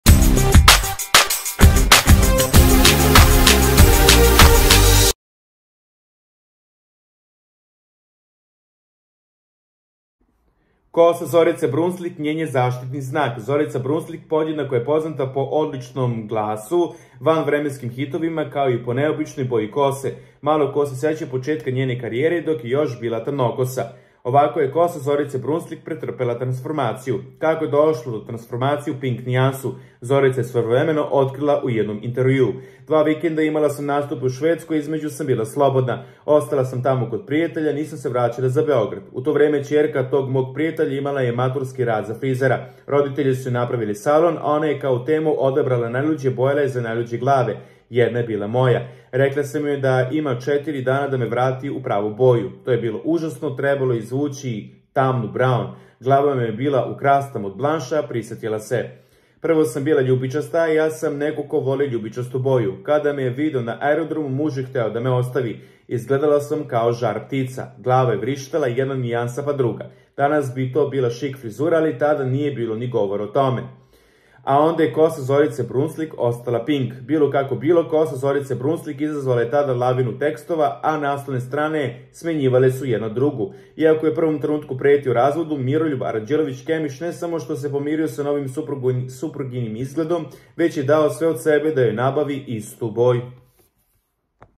NAKON OVOGA SE NIKAD NIJE VRATILA BRAON BOJI! Ovako je kosa Zorice Brunclik postala pink
Kosa Zorice Brunclik njen je zaštitni znak